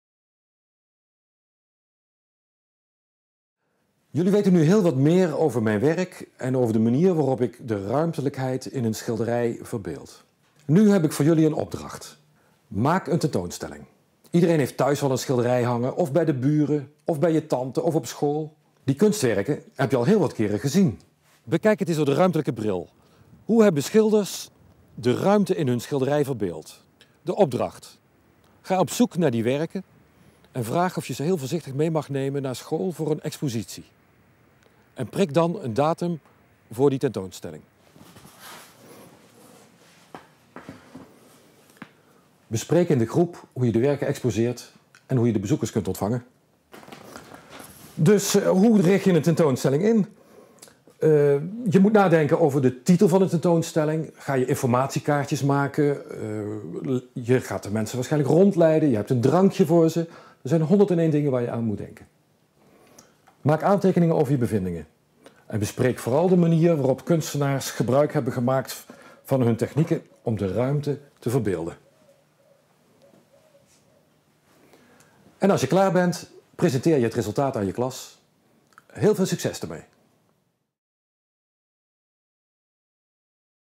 Kunstopdekaart_Verdiepingsopdrachtruimte_Kunstbalie
Verdiepingsopdracht in het thema Ruimte, horende bij het spel Kunst op de Kaart. Heeft u interesse in meer informatie over dit spel?